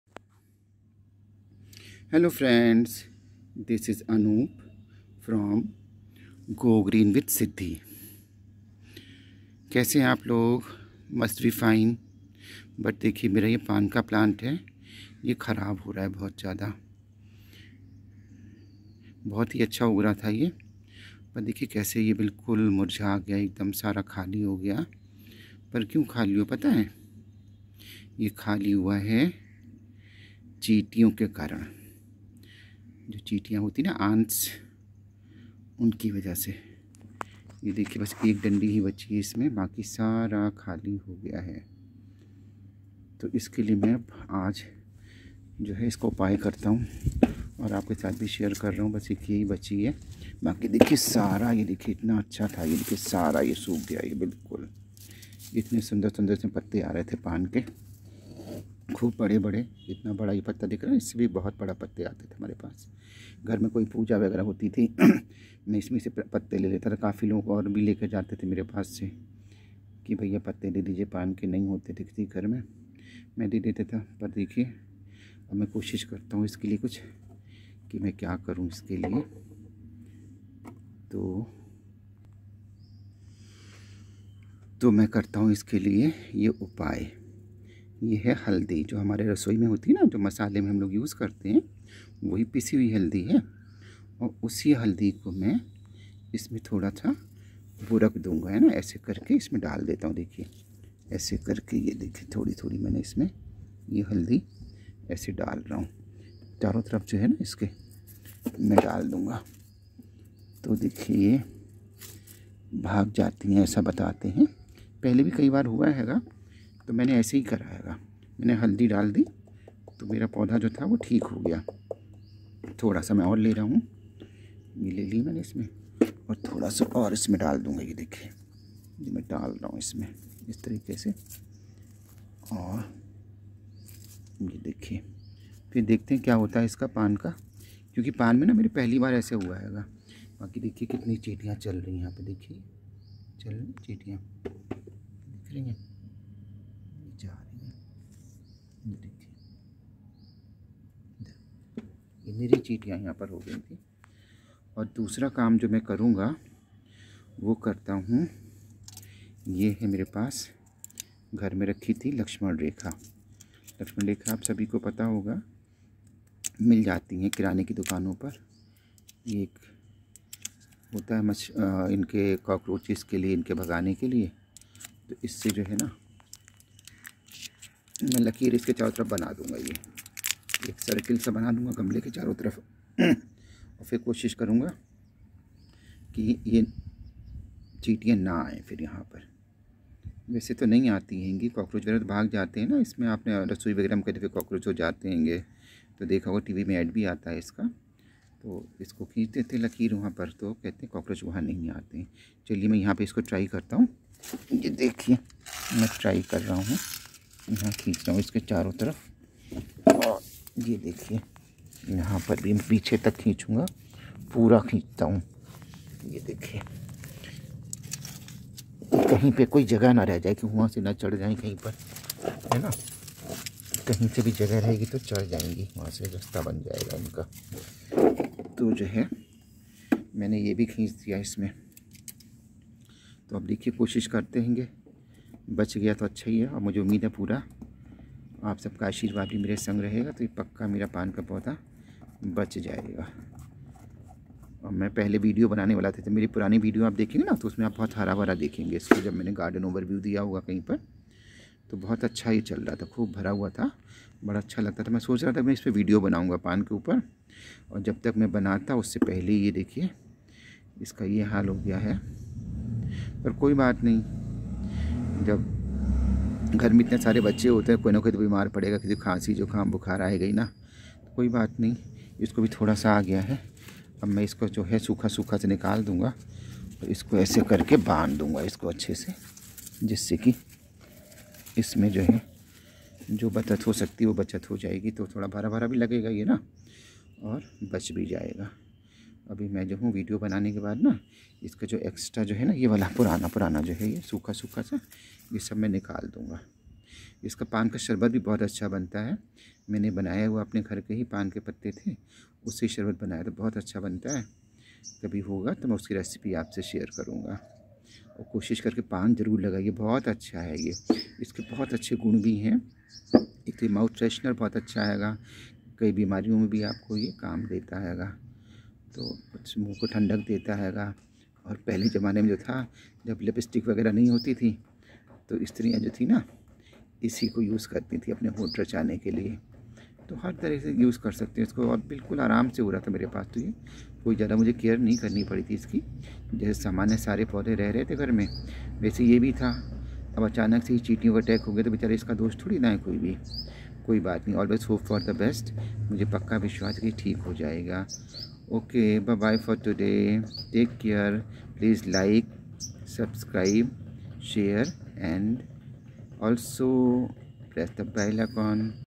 Naturally Remove ANTS from Plants and Soil | Tips to Protect Plant from ANTS
In this video, we will talk about protecting our plants and soil from Ants and other insects using some basic homely ingredients like Haldi, Laxman rekha and so on.